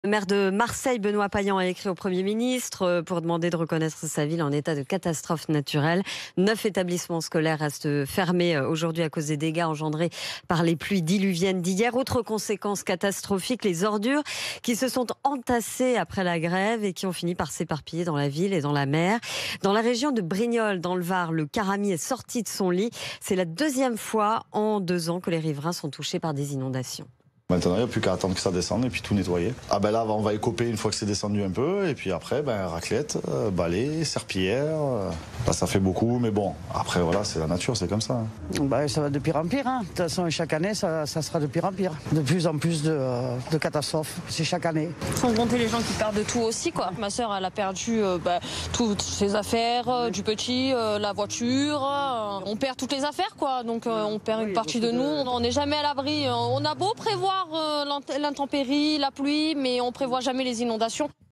Inondations dans le Var: la région de Brignoles se réveille les pieds dans l'eau
"Ma sœur, elle perdu toutes ses affaires, sa voiture. Chaque année, ça sera de pire en pire": après les intempéries, la région de Brignoles dans le Var est toujours sous les eaux